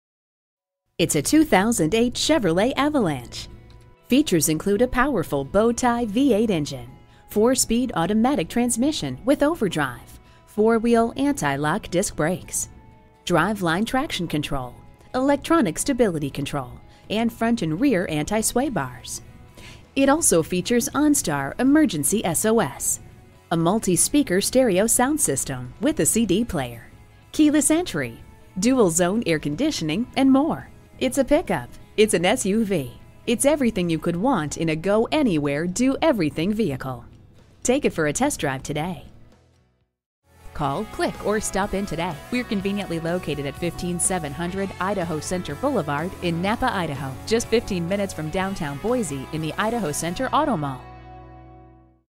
2008 Chevrolet Avalanche Nampa ID Boise, ID 43494B - SOLD
Year: 2008
Make: Chevrolet
Model: Avalanche
Engine: 8 Cyl. 5.3L
Transmission: Automatic
Color: White
Mileage: 82002
Stock #: 43494B
OEM Certified: S. If you've been looking to find just the right 2008 Chevrolet Avalanche 1500, then stop your search right here. This is the perfect, do-it-all truck that is guaranteed to amaze you with its adaptability to all of your needs. This Avalanche 1500 is nicely equipped with features such as 4-Speed Automatic with Overdrive and 4WD. New Car Test Drive called it ...a superb truck when it comes to flexibility for hauling cargo and people. It can be converted from pickup to SUV in a couple of minutes. And it's a superb vehicle for towing. From ride and handling to looks and packaging, it's a refreshing upgrade of an innovative concept that delivers unmatched flexibility without sacrificing function... Don't get stuck in the mudholes of life. 4WD power delivery means you get traction whenever you need it. Designated by Consumer Guide as a Recommended Large Pickup in 2008.

Edmark Superstore
Address:
15700 Idaho Center Blvd